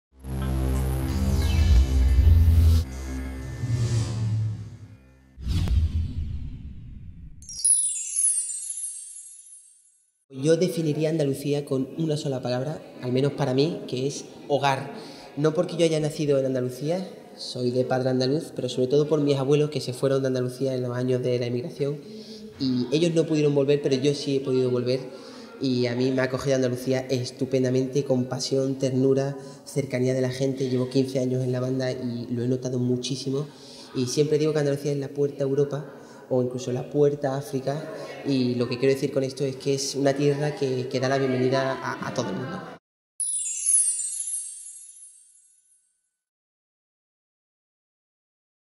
HDL El granito de luz del presentador Felipe Delgadillo
Para definir Andalucía, Felipe Delgadillo tiene claro qué palabra utilizar: "Hogar". A pesar de no haber nacido en Andalucía, así lo siente por influencia, principalmente, de sus abuelos: "Se fueron de Andalucía en los años de la emigración. Ellos no pudieron volver, pero yo sí, y me ha acogido estupendamente. Con pasión, ternura, cercanía...". Y eso es algo que el presentador ha notado en todos los años que lleva al frente del programa La Banda. Y añade: "Andalucía es la puerta a Europa, e incluso la puerta a África. Y lo que quiero decir con esto es que es una tierra que da la bienvenida a todo el mundo".